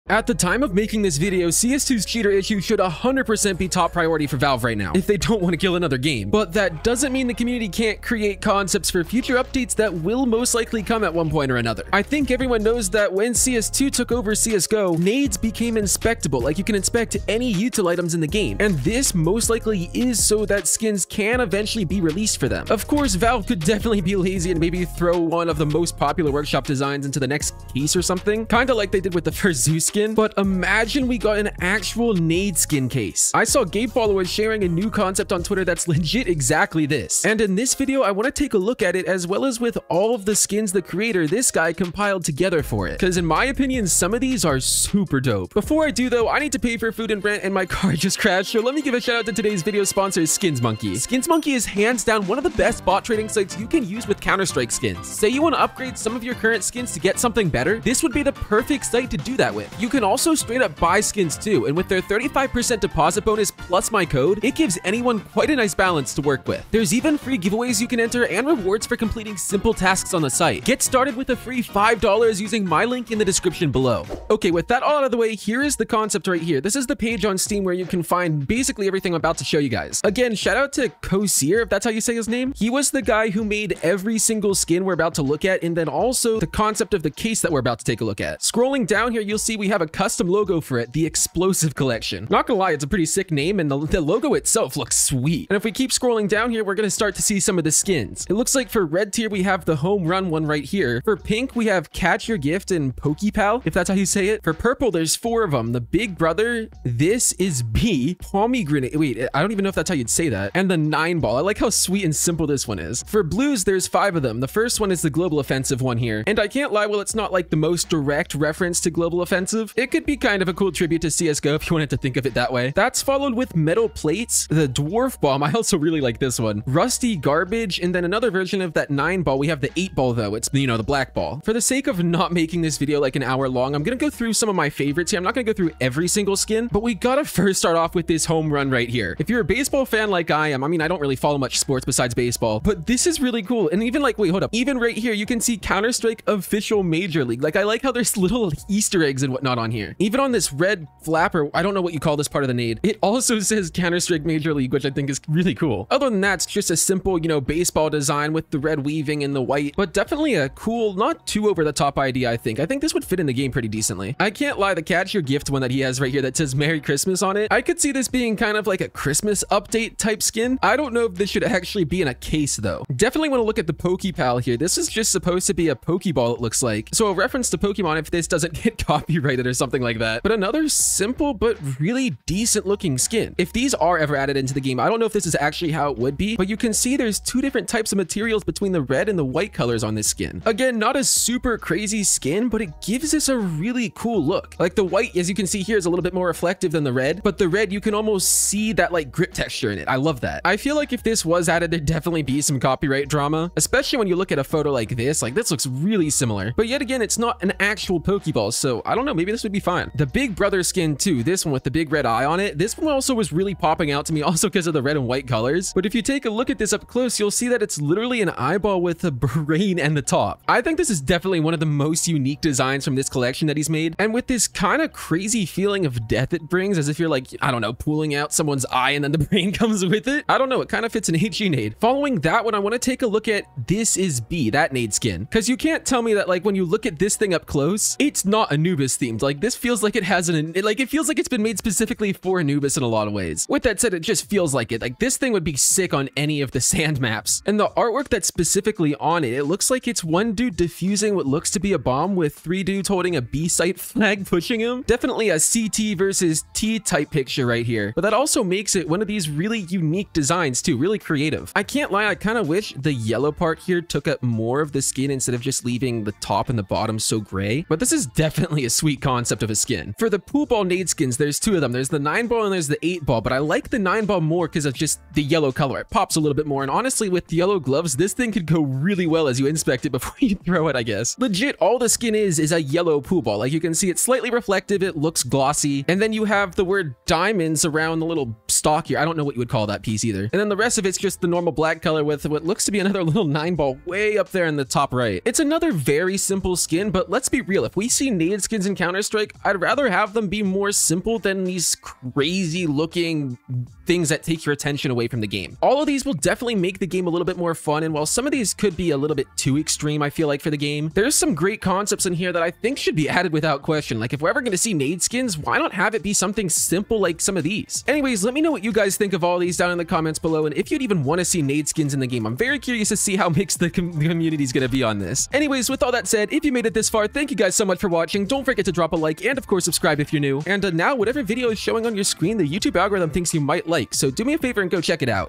NEW CS2 NADE SKINS CASE CONCEPT!!
Here's a super cool concept for a cs2 nade skin collection...not coming anytime soon but this is what something like this could look like!!

YOUTUBE THIS IS NOT GAMBLING!!

thx for watching love you all😘